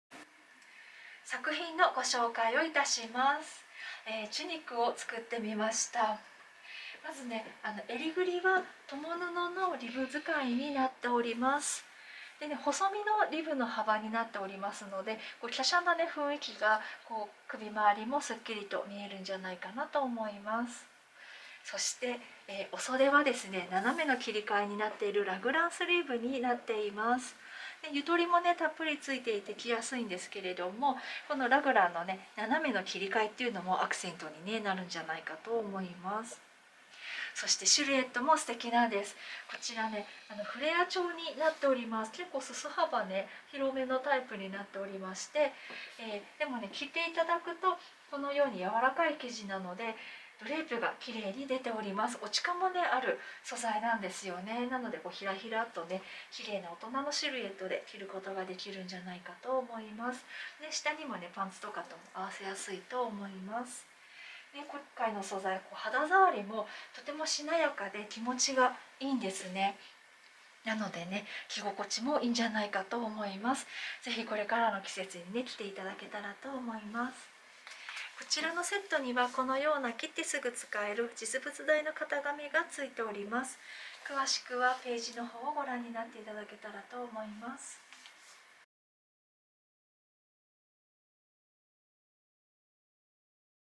QW0074-1--SET
【通販品番：QW0074-1--SET】【ソーイングセット】ラグランスリーブの共布リブAラインニット

一枚あると便利なラグランスリーブのAラインニット。
衿ぐりは、共布仕立てのリブですっきりとしたデザイン。
斜めラインが特徴のラグランスリーブで、着やすく動きやすいシルケットです。